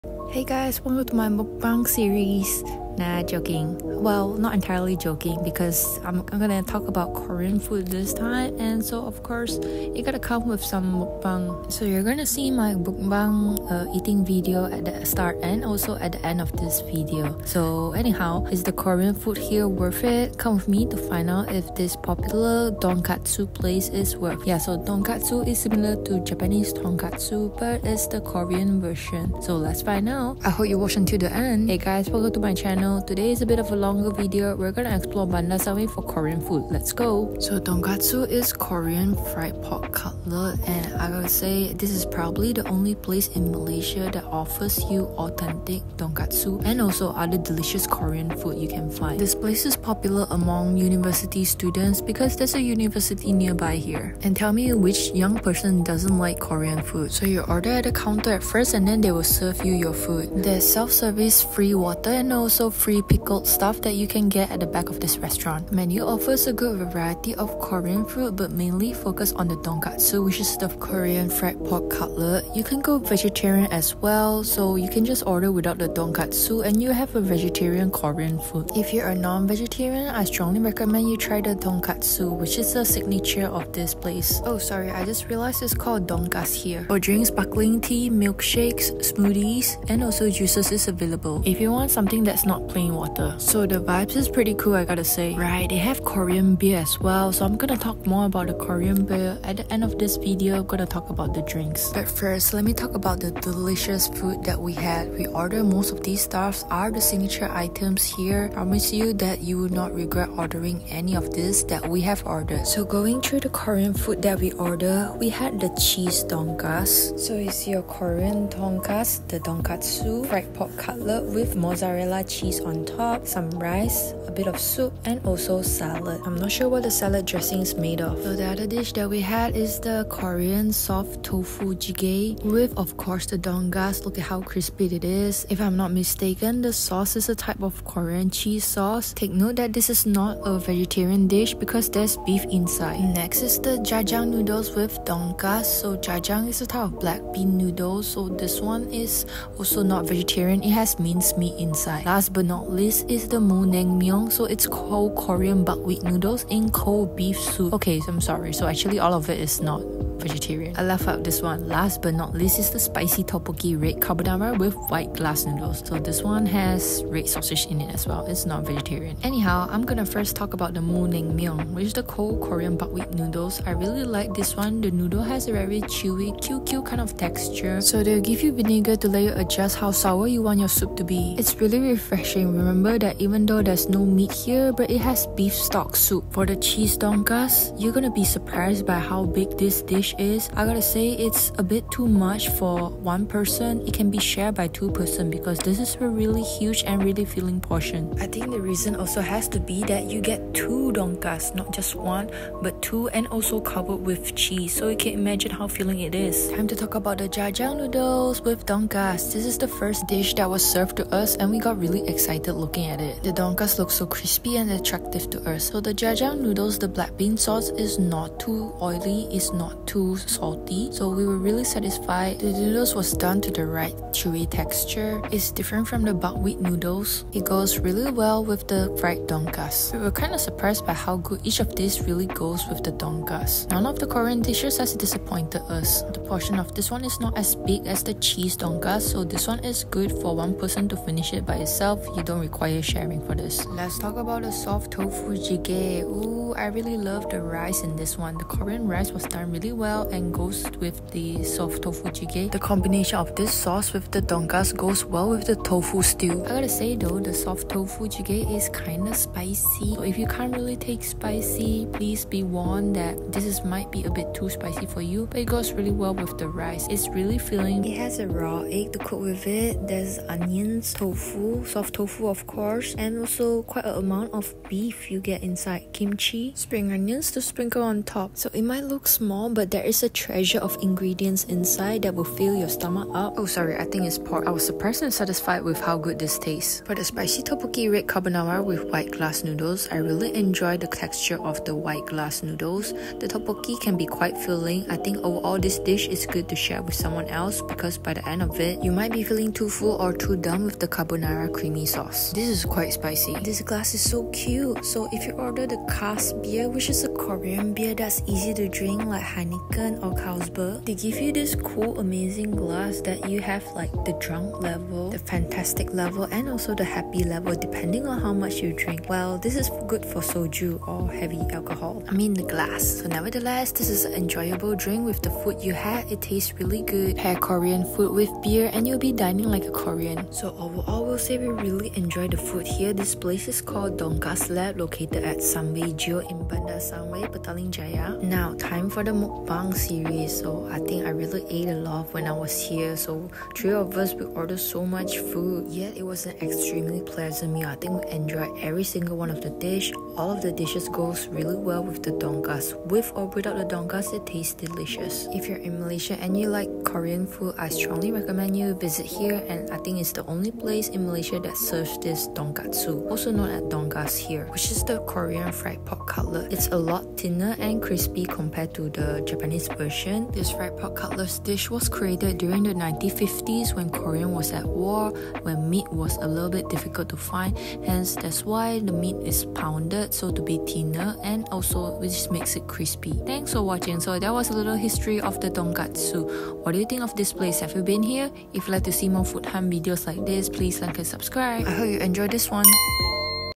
MALAYSIA + MUKBANG : the only korean restaurant with the best donkas / donkatsu
Donkas Lab is the first in Malaysia that gave us this korean delicacy in KL. The amount served for each portion is plentiful enough to satisfy any hungry big eaters.

Non-halal  Vegetarian-friendly

Dongaseu  /Donggasu / Donkkaseu /dontkatsu  is a korean version of the more popular Japanese fried meat cutlet that we know of, tonkatsu or the European schnitzel.

Soft Tofu Jigae (RM 33, with donkas)
Cheese Donkas (RM 33)
Jajang Rice (RM 28, with donkas)
Spicy Topokki Red Carbonara with Wide Glass Noodles (RM 22, without donkas)
Mul Neng Myun (RM22)
cold korean buckwheat noodle in cold beef soup is very refreshing with adjustable vinegar to pour in the soup.

Cass Beer (RM 28)
Good donkatsu without a nice korean beer? You gotta be kidding me!

Aside from the signature donkatsu, the korean dishes were all wonderful, be warn with the spicy topokki, it can get really spicy, lvl 2 was more than enough spicy for us.

Crispier breading than your average mediocre schnitzel or more pounded thinner version of your usual tonkatsu, Donkas lab is a korean place you won't regret visiting.

Note: Self-serving water and pickled goods are available!

This koreanized version of the tonkatsu is an iconic dish in Korea during 1960s post war, the different is that the pork meat is pounded, thinner compared to tonkatsu ( as back then post war meat was scarced and pricy),